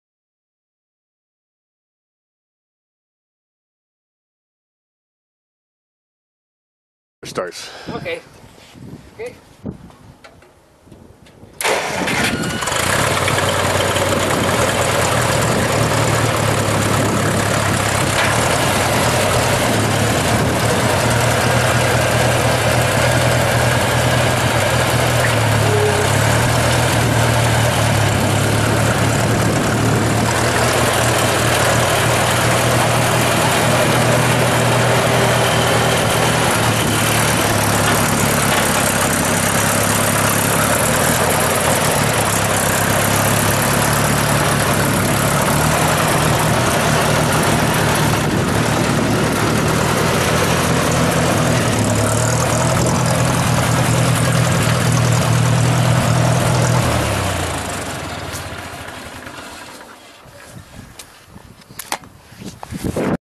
1959 Massey Fergusen 65 tractor for sale | sold at auction March 11, 2015
1959 Massey Fergusen 65 tractor
3,872 hours on meter
Continental 2.9L four cylinder gas engine
33 HP
Manual transmission
6F-2R gears
Three point
Draw bar
540 PTO
Du-Al loader
84" bucket
Three tine grapple fork
Single rear hydraulic remote
13 gallon fuel tank
Wheel weights
11L-15 front tires
15.5-38 rear tires
Engine has a misfire
Three point issues
Power steering inoperable
Serial 670250

It was located in Quinn, SD, and we sold it to the highest bidder for $1,980.